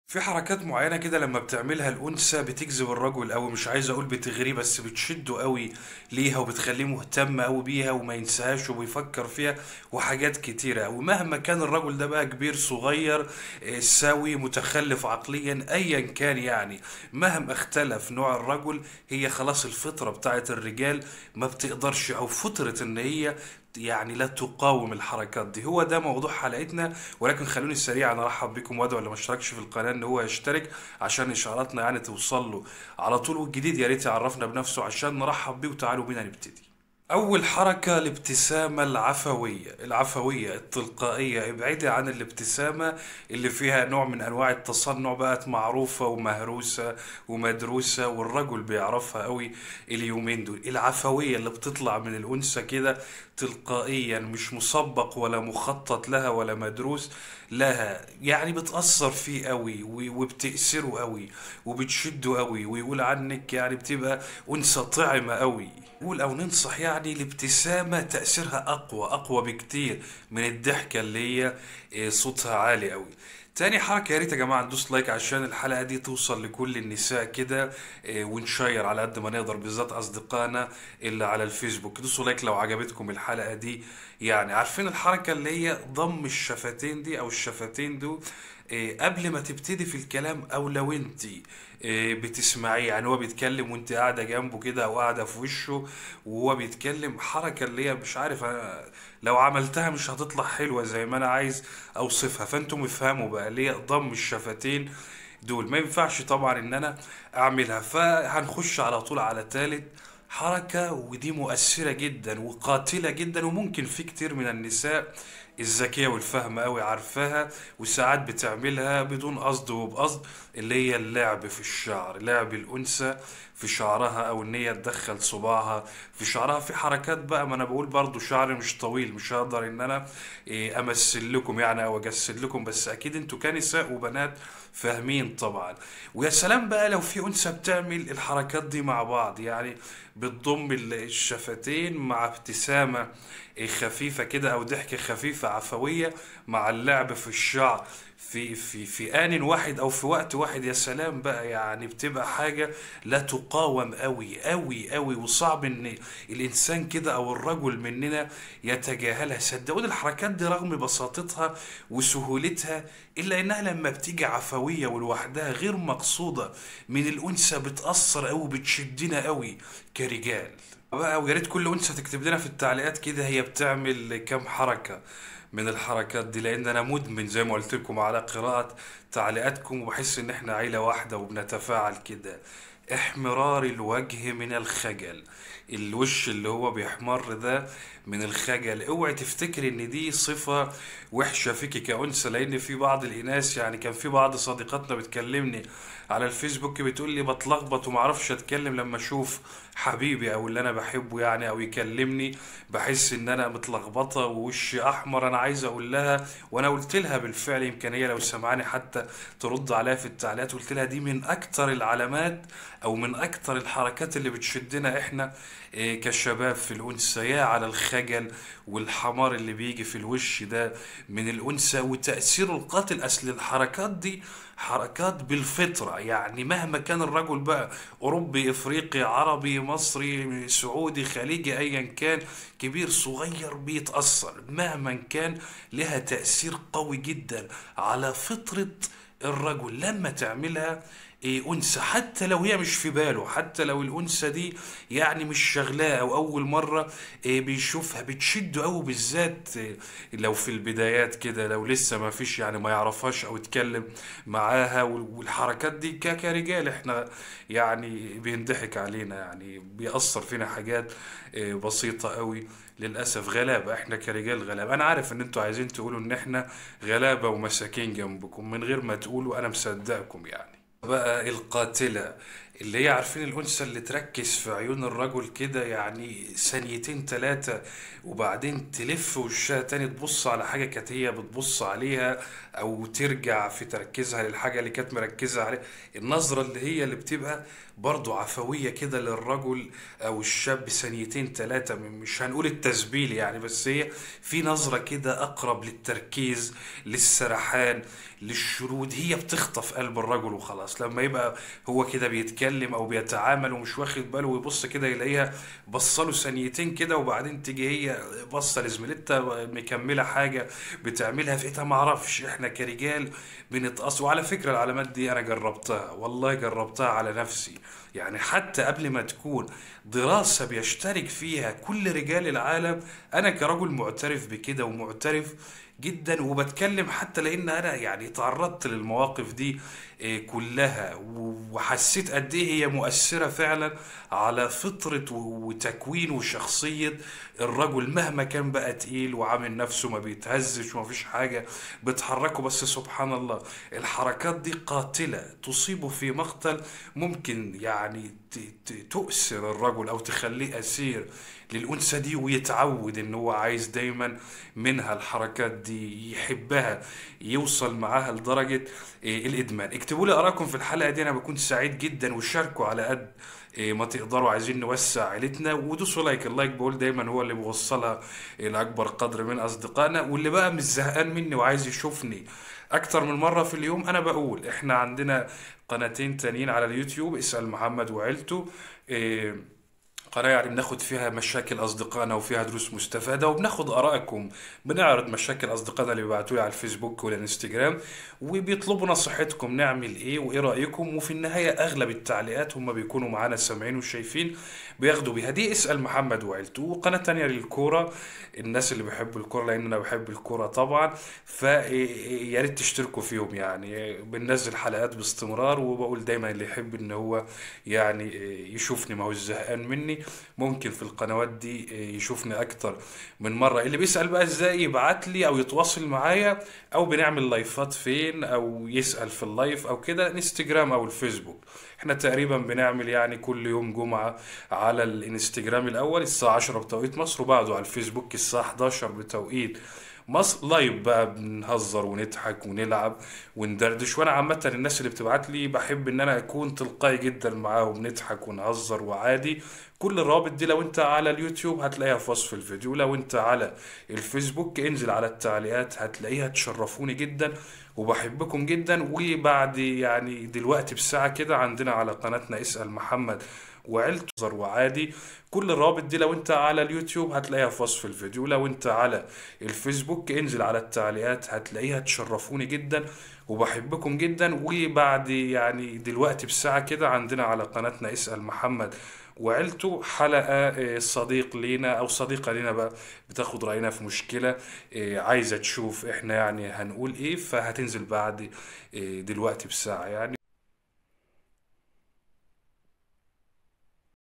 حركات عفوية بالجسد إذا فعلتها المر-أة يقع الر-جل اسير فى يديها♥️
صفحتنا عالفيسبوك علشان تشوفوا فيديوهتنا وتحضرو الايف معانا

_link